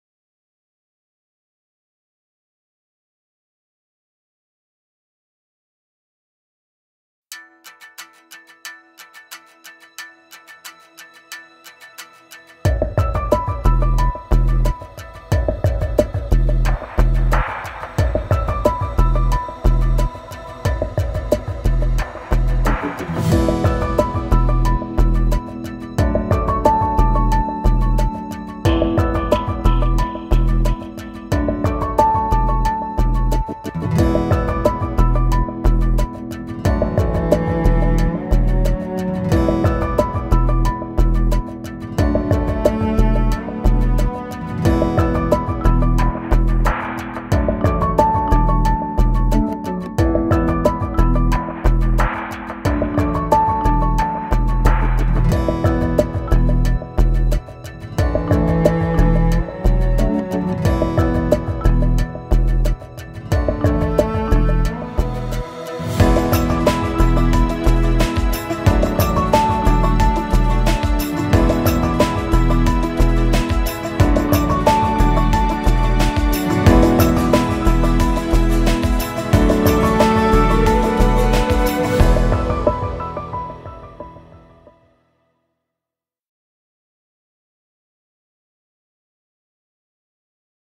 CorDEX ToughPIX II Trident Explosion Proof Digital Camera Promo | 247able.com
CorDEX ToughPIX II has now been superseded by CorDEX ToughPIX II Trident Explosion Proof Digital Camera.

The ToughPIX II Trident Edition is available to buy online at 247able.com.